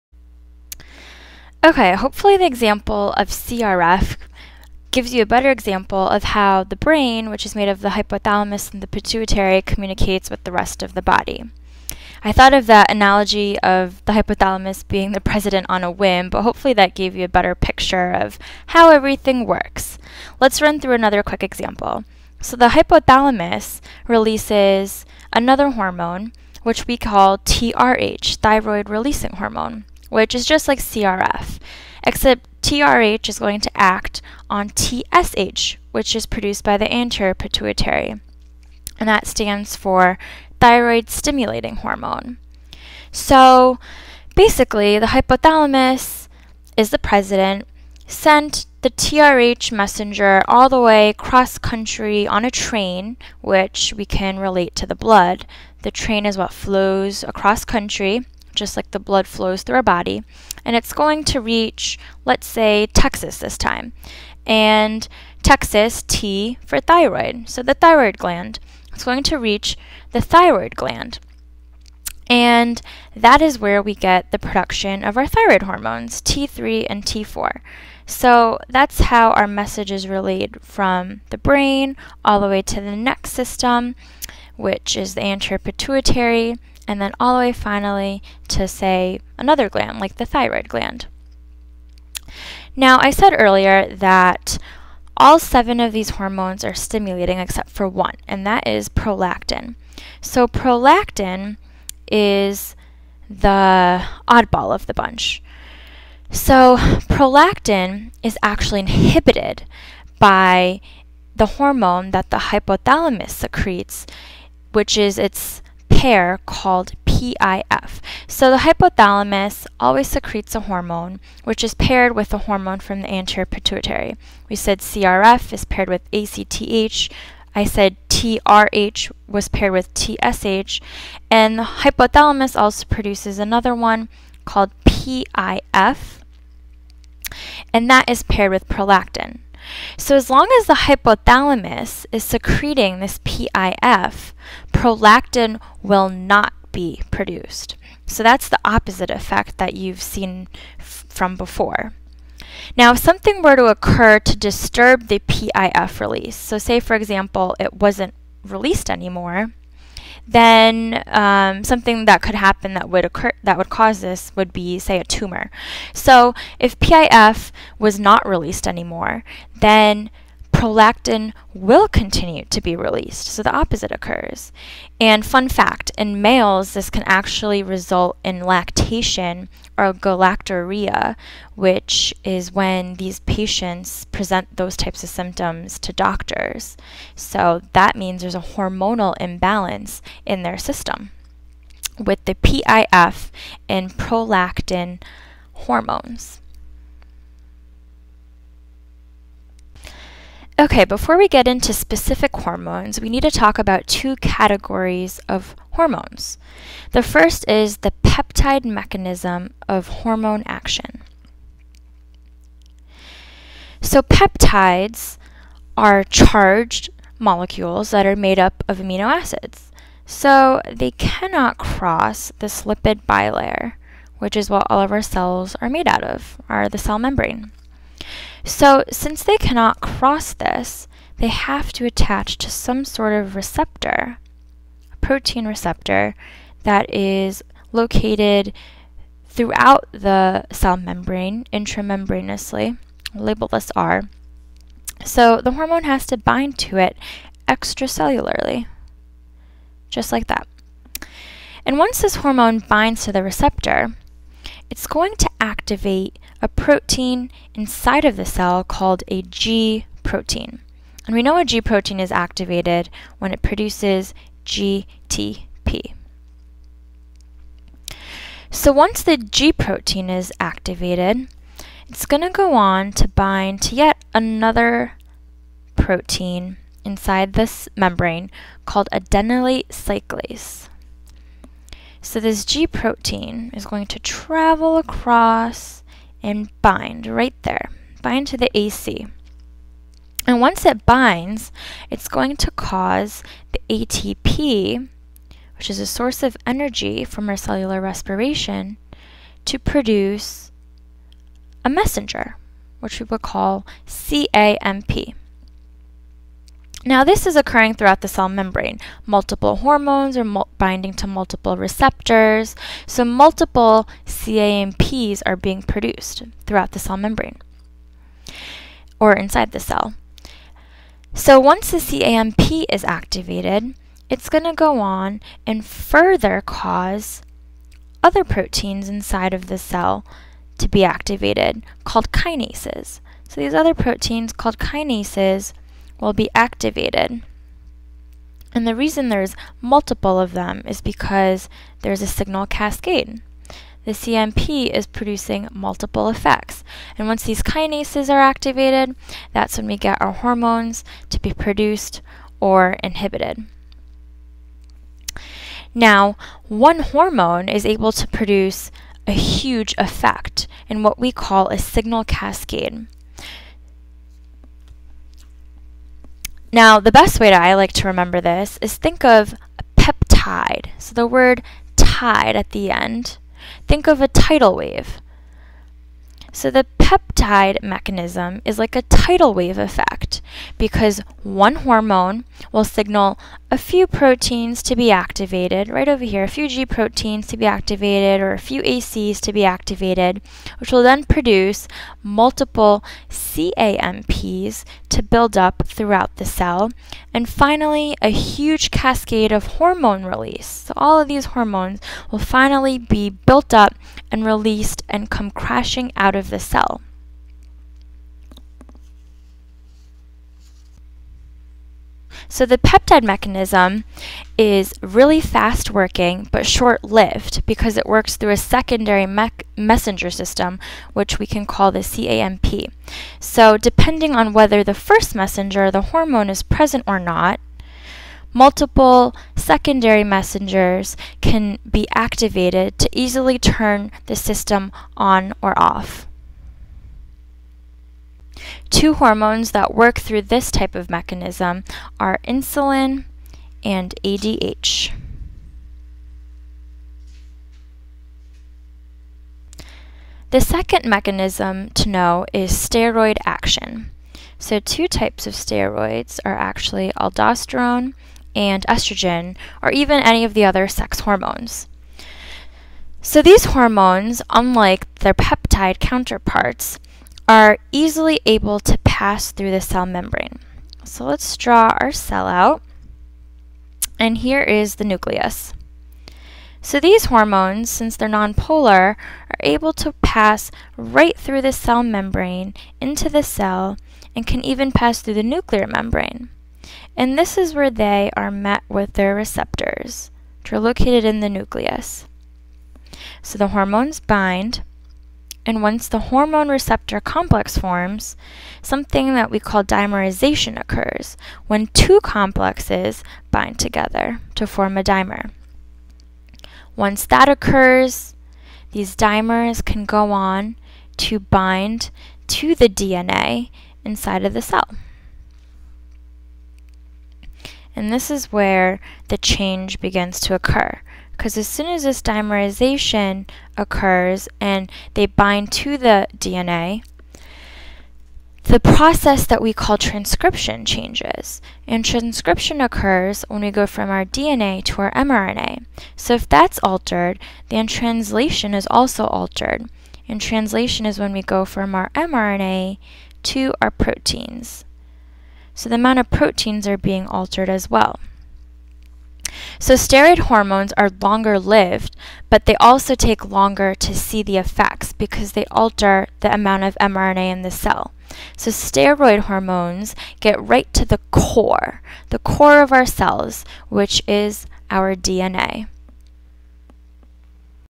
Endocrine System Part 2
Topics: Negative Feedback and Peptide/Steroid Mechanisms of Hormone Action.